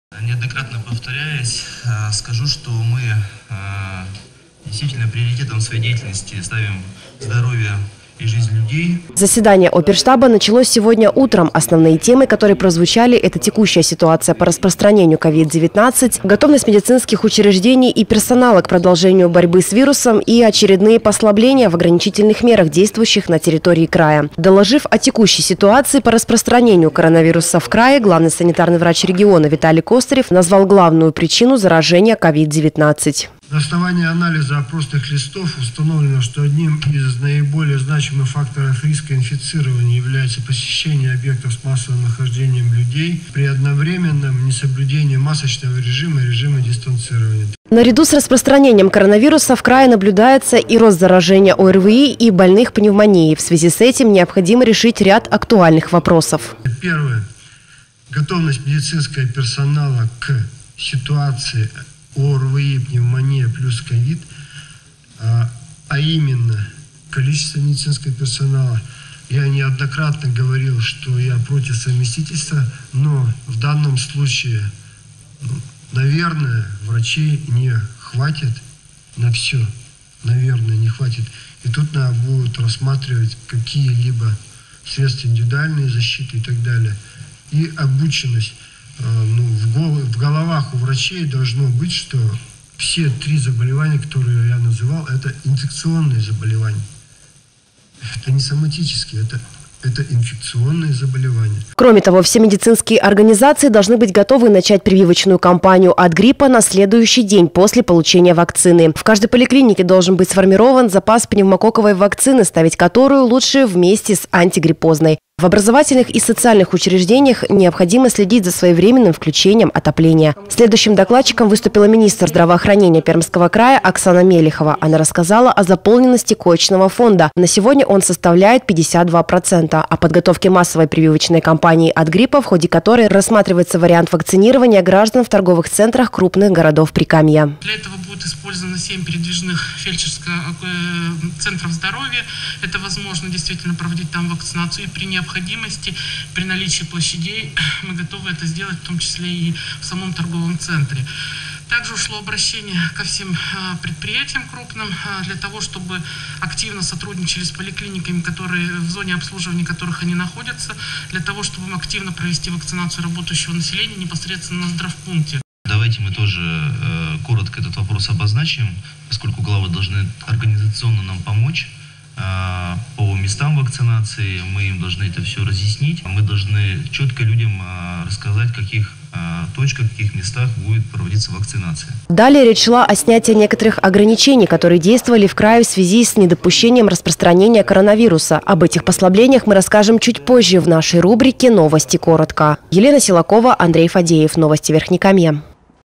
Заседание оперштаба
Вчера утром состоялось очередное заседание регионального оперативного штаба по борьбе с распространением новой коронавирусной инфекцией. Чиновники обсудили текущую ситуацию и приняли ряд важных решений.